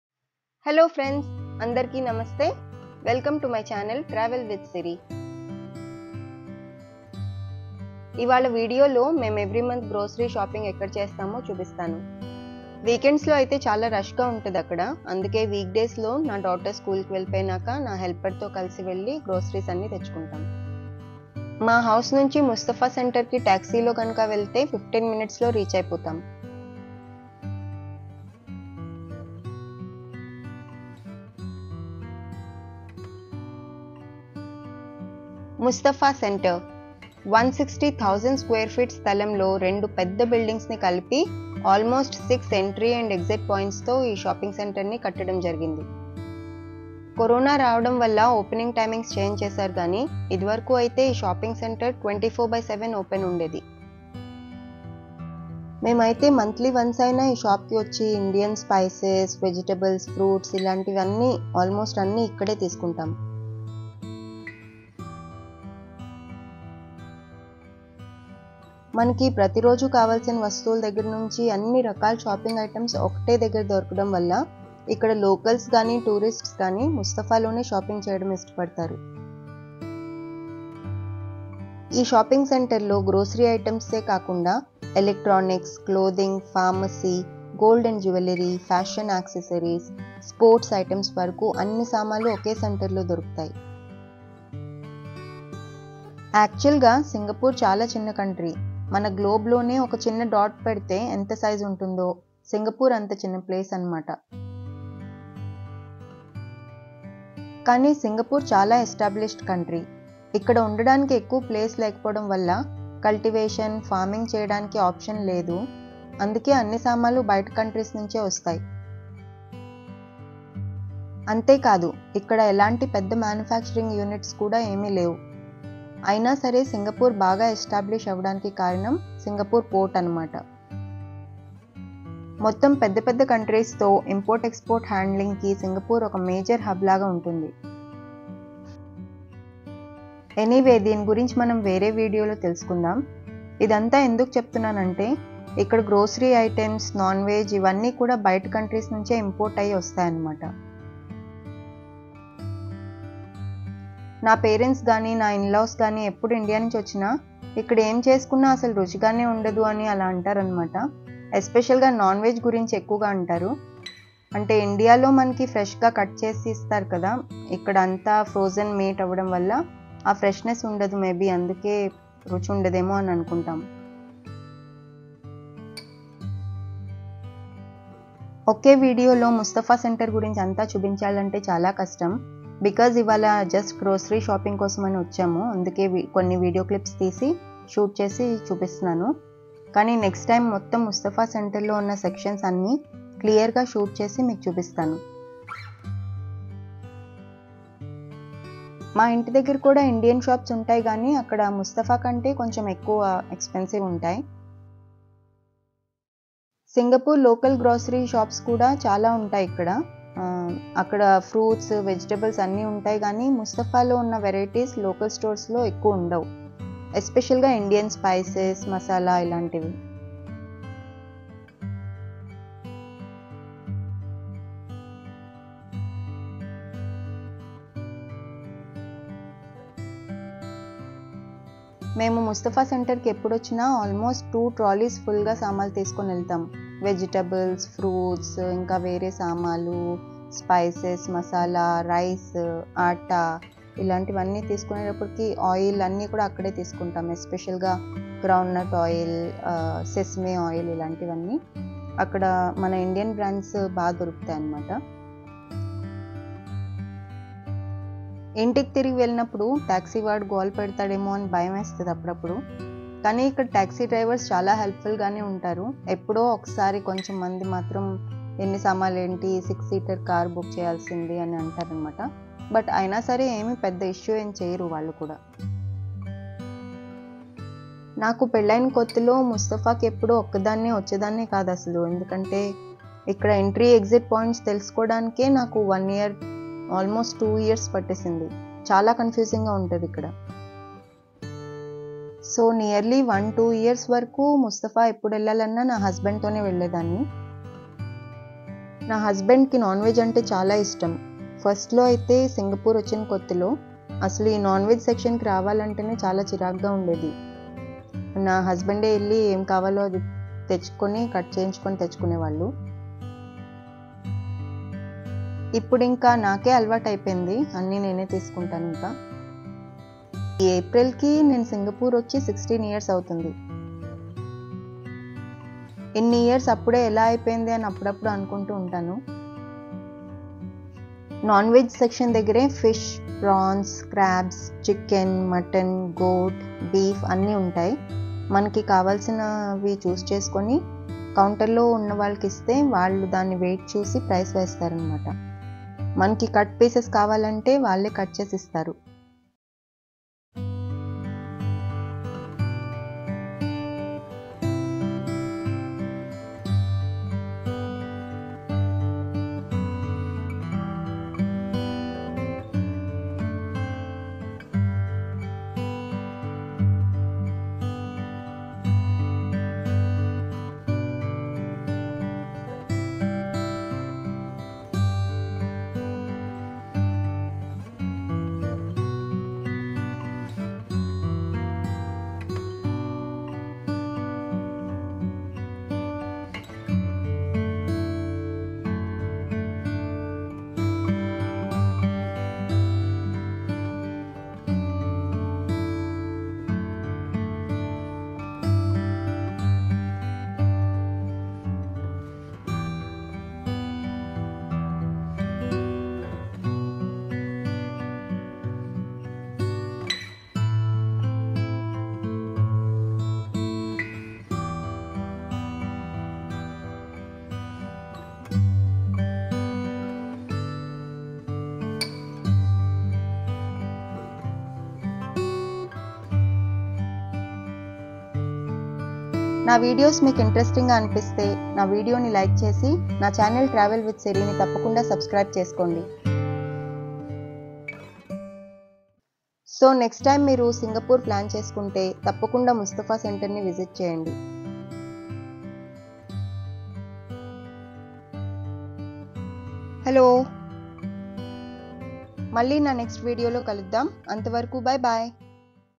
Podcast#4 Mustafa Shopping Centre Grocery Shopping Singapore Telugu Vlog 2023
Mustafa Shopping Centre Grocery Shopping Singapore 2023

Ivala video lo memu every month grocery shopping ekkada chestamo chupistanu.

Mustafa centre ani Singapore lone chala pedda shopping centre.